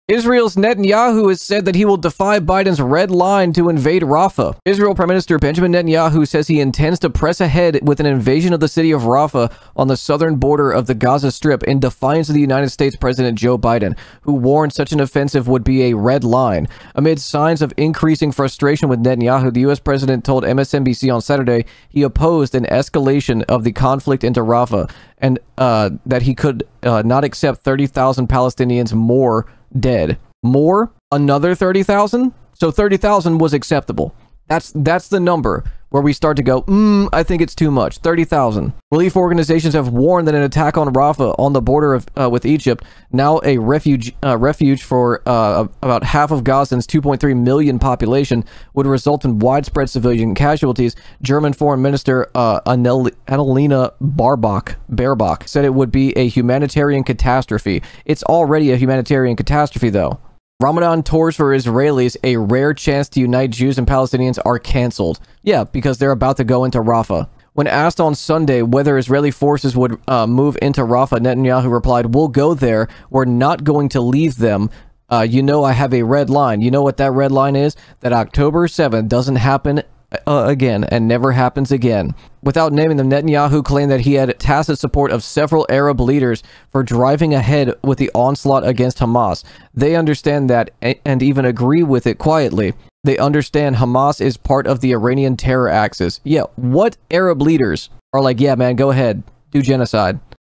Israel Has no Red Lines
Biden sets ‘Red Lines’ and Israel has none.
with gremloe